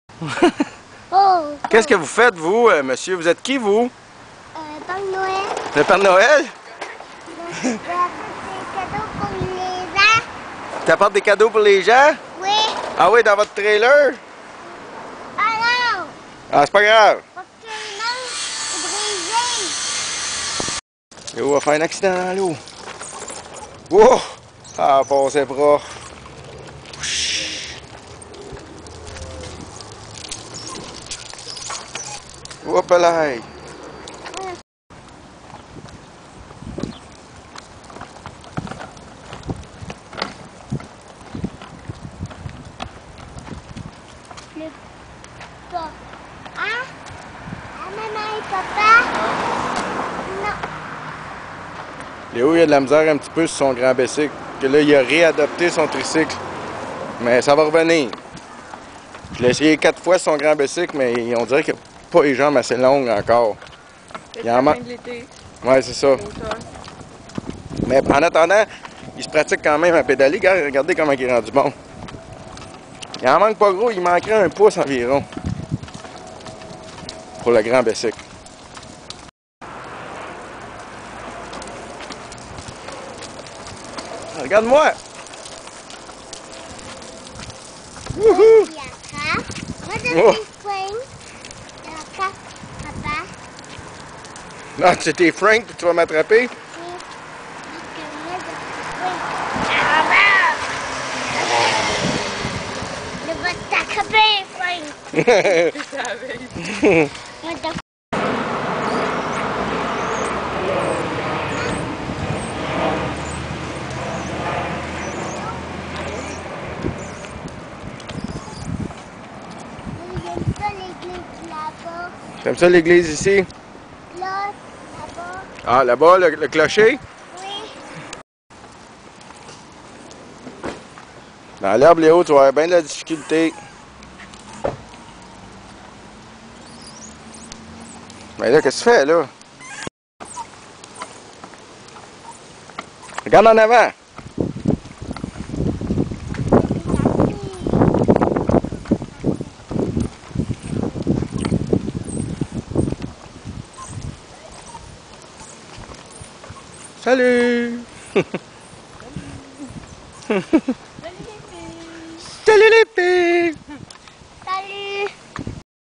Léo en tricycle
La nouvelle bicyclette de Léo est juste une petite affaire trop haute pour lui. Mais grâce aux séances d'apprentissage avec son papa, Léo est devenu assez bon pour pédaler sur son tricycle. Au moins, il se pratique à pédaler! Il sera juste meilleur sur sa bicyclette lorsqu'il aura grandi un petit peu.